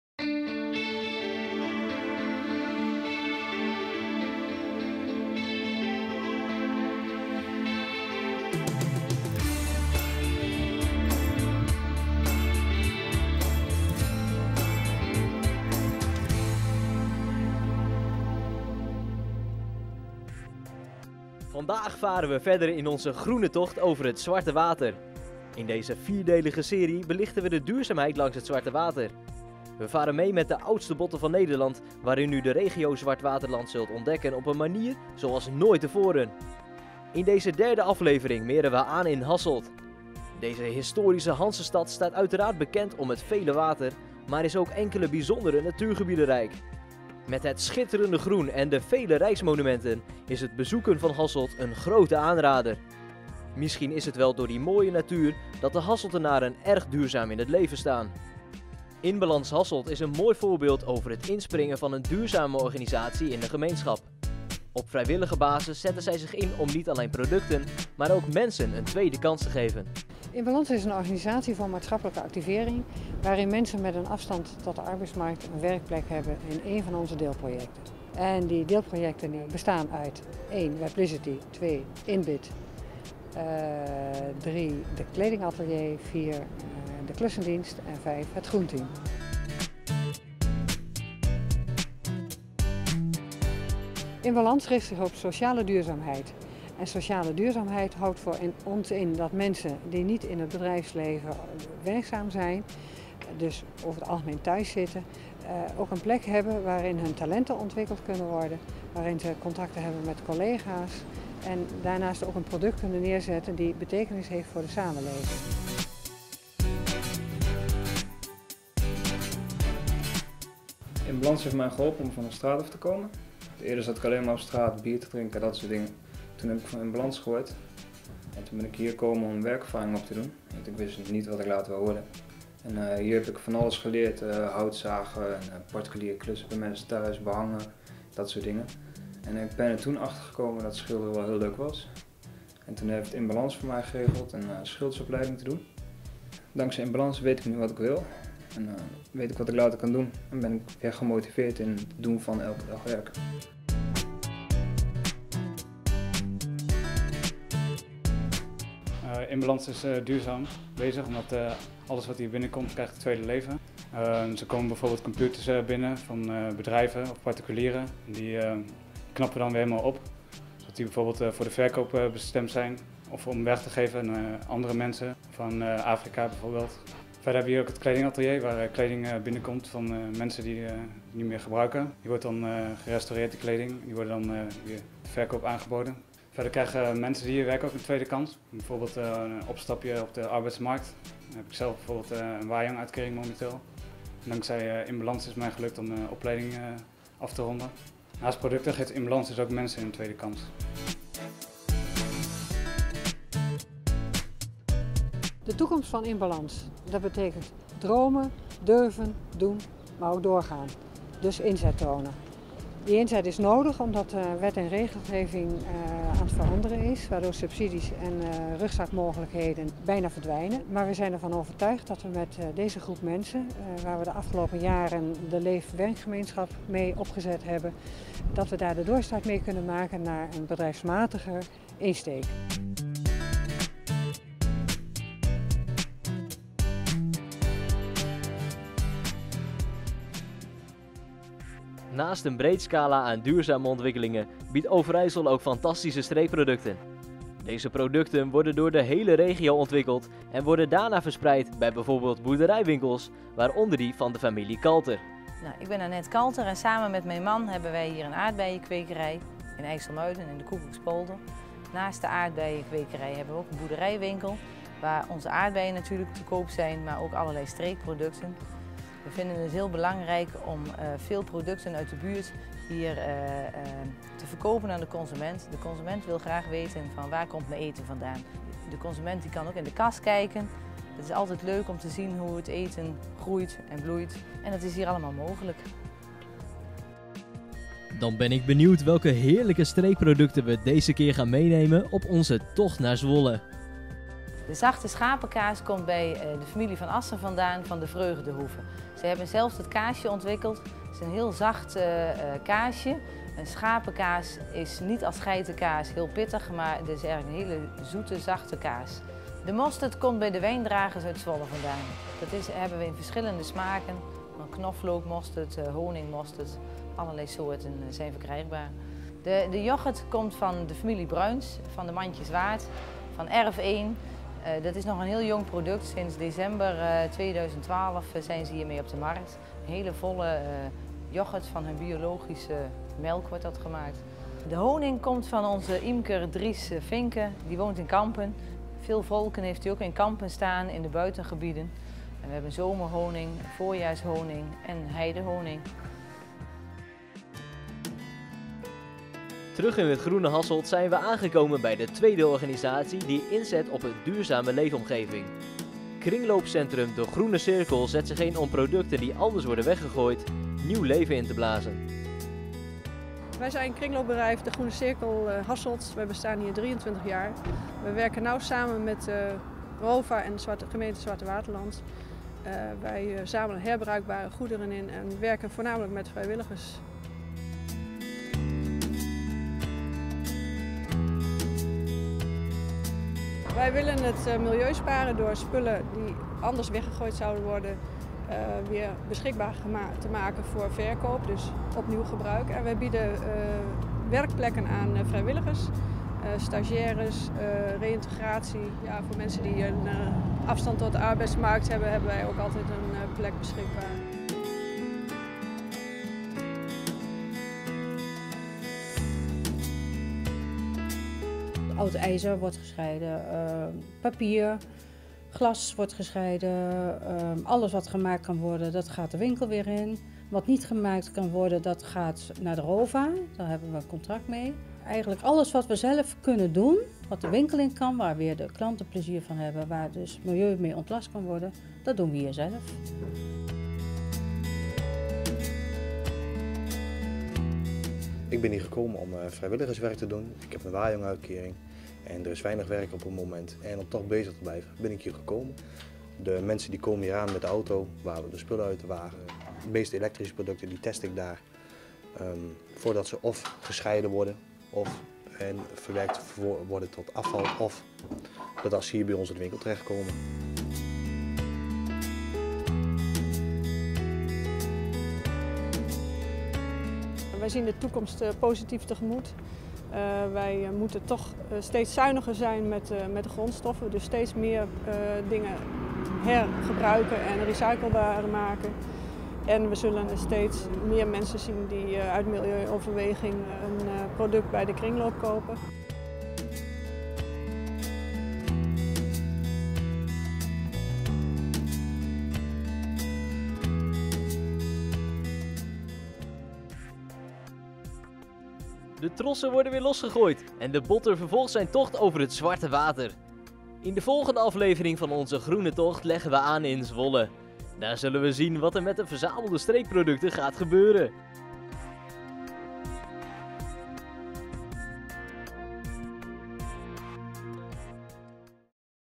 De Groene Stroom - Aflevering 3
In deze tocht met de oudste botter van Nederland over het zwarte water belichten de duurzaamheid langs het zwarte water. U zult de regio Zwartewaterland ontdekken op een manier zoals nooit tevoren.

Aflevering 3 - Hasselt:
- In Balans Hasselt
- Kringloopcentrum De Groene Cirkel

2013 © Jesper Kroondijk en Jeroen Ganzevles
Afstudeerproject stage Productiehuis Landstede